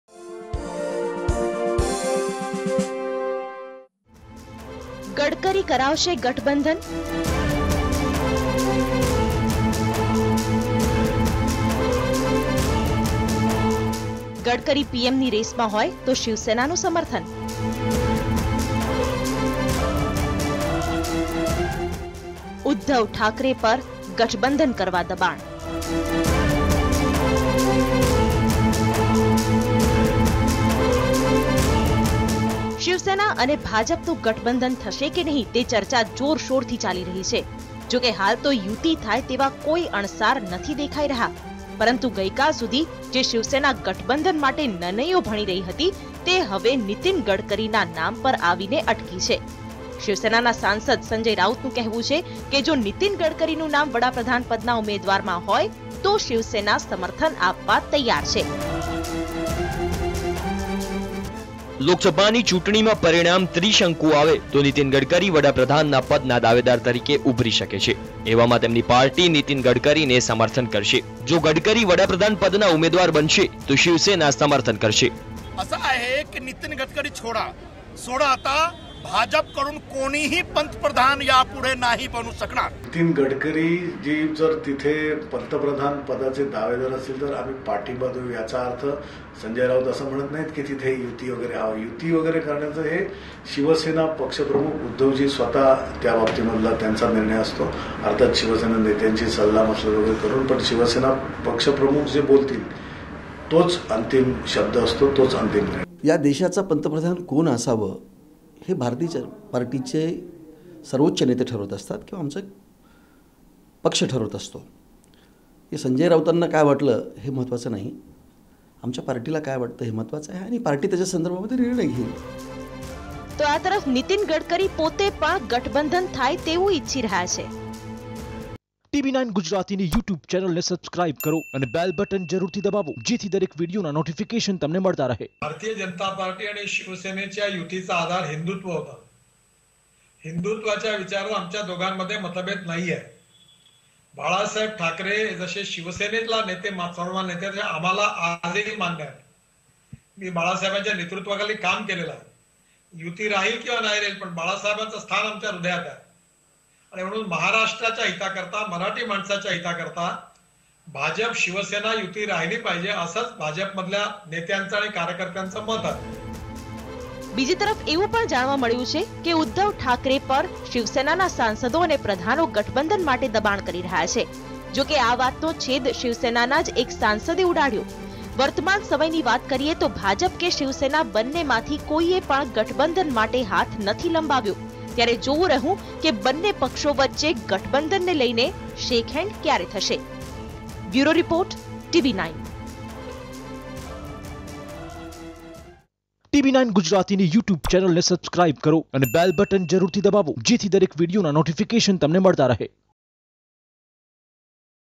Shiv Sena will support Nitin Gadkari if he emerges as PM face: Sanjay Raut- Tv9
Shiv Sena will support Nitin Gadkari if he emerges as PM face: Sanjay Raut